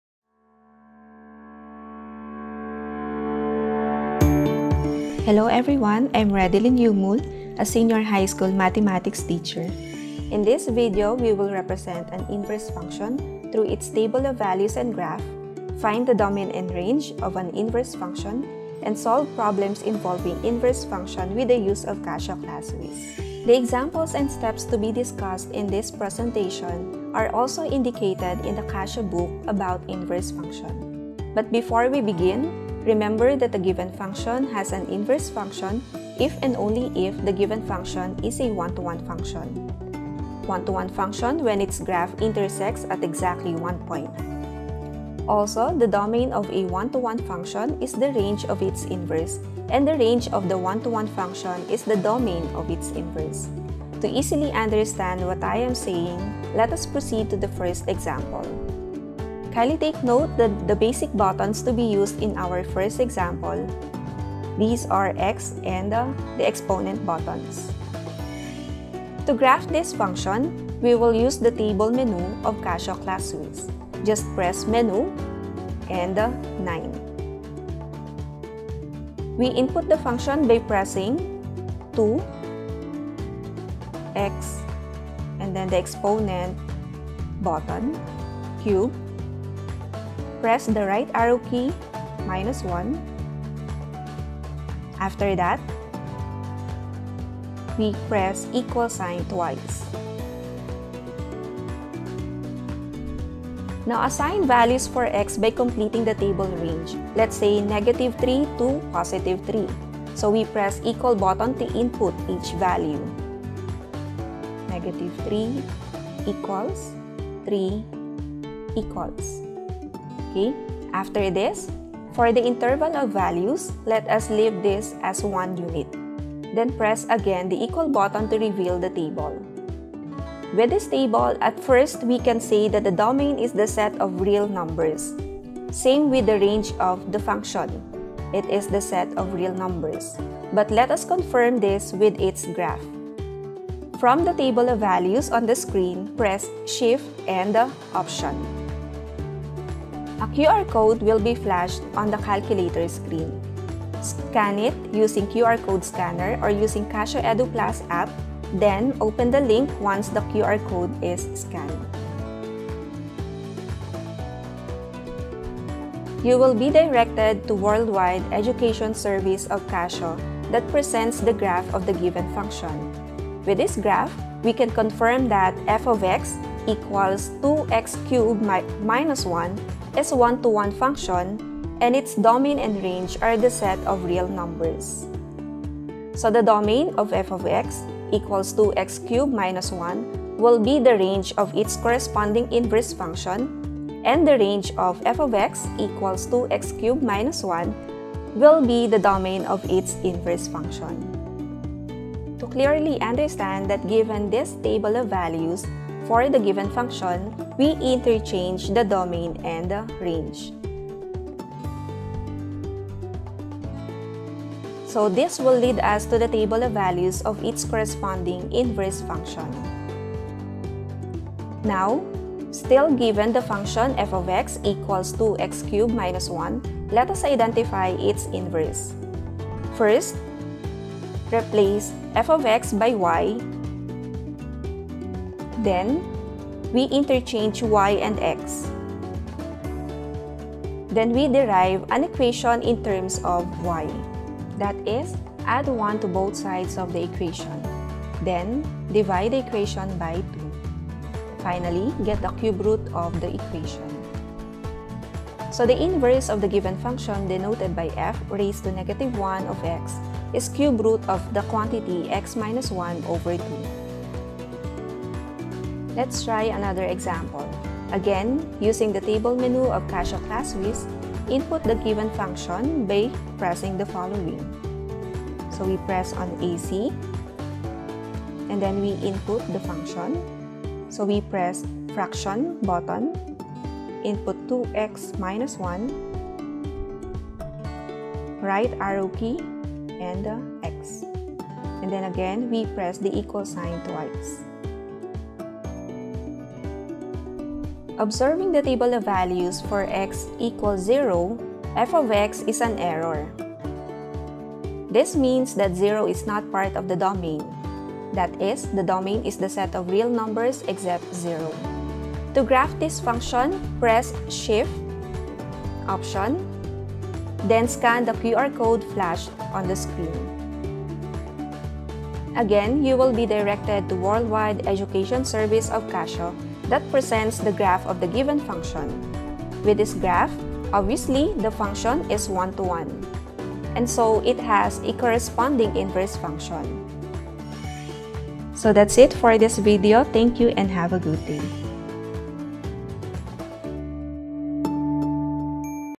Casio Classwiz: Grade 11: "Inverse Function"
Join Ms. Redelyn Yumul as she explores Inverse Function with Casio Classwiz!

We provide quality trainings for teachers & students plus more!